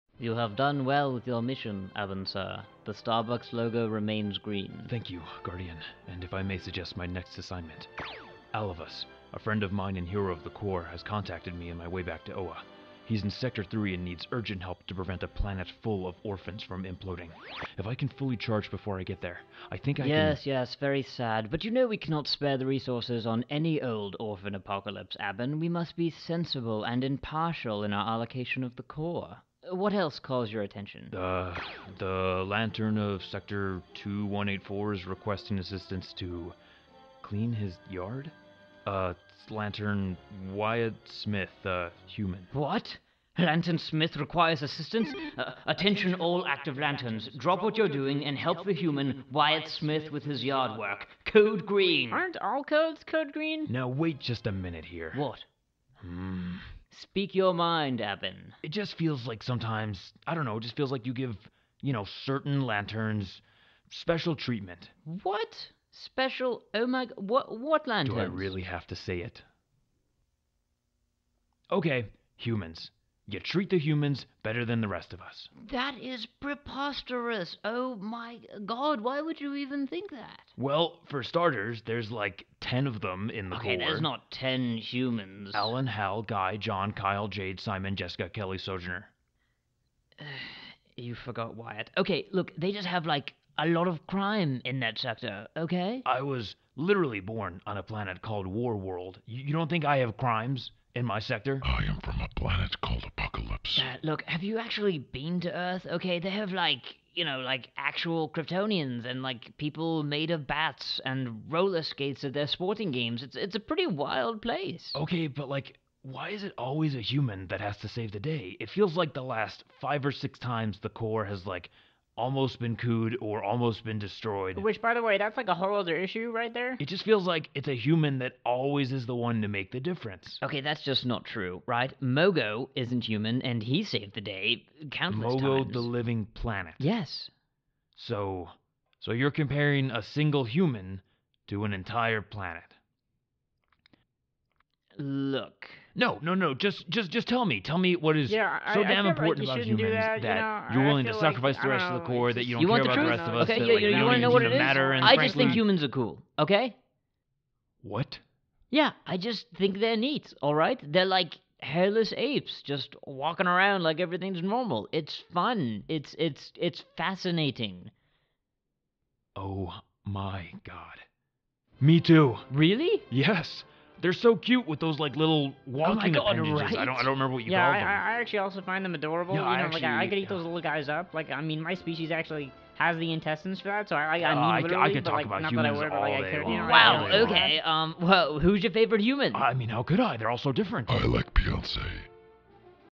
The Problem With Green Lanterns
The Guardians of Oa come face to face with their worst enemy - prejudice!

All characters voiced by @carterfromcyberlife